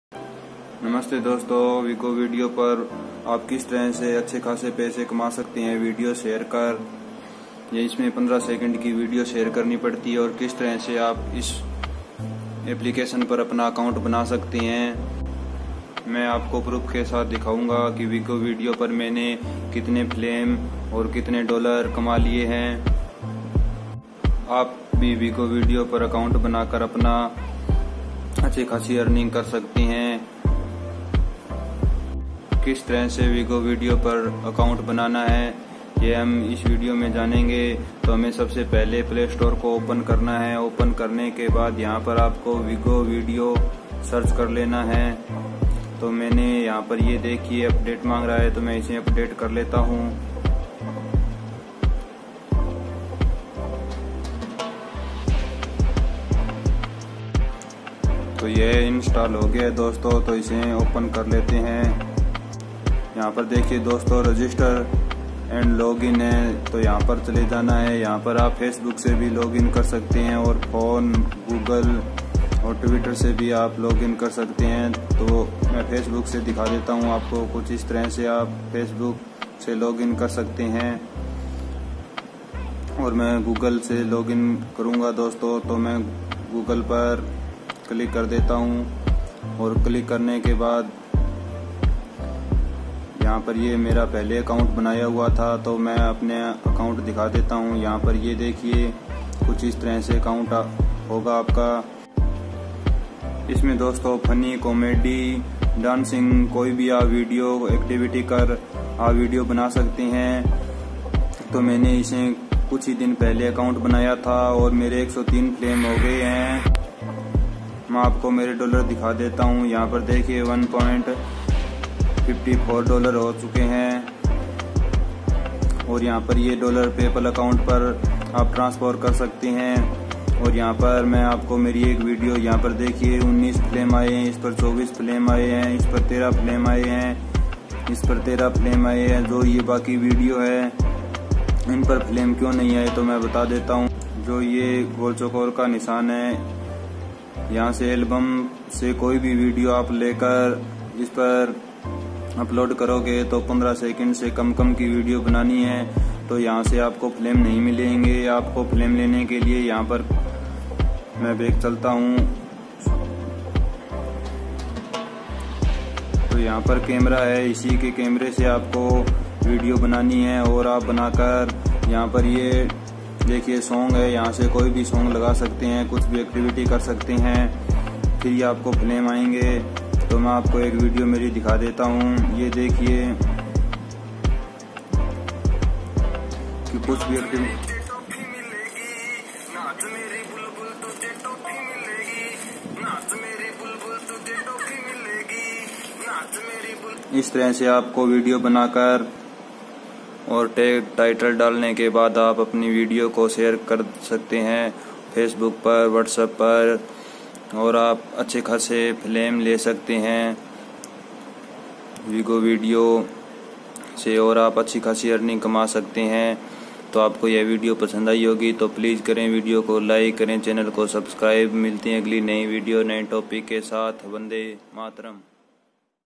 How to create a account on Hypstar/Vigo video | earn money Hypstar?
Hello friends In this video, I have talked about the Vigo video application, how you can earn money from Vigo Video and how to create an account and how to increment the form from Vigo Video vigo video par video upload kaise kare step-by-step bataya hai, vigo video ko kaise update kare

How to use Vigo video aap
How to install Vigo video aap in playstore

Vigo video App download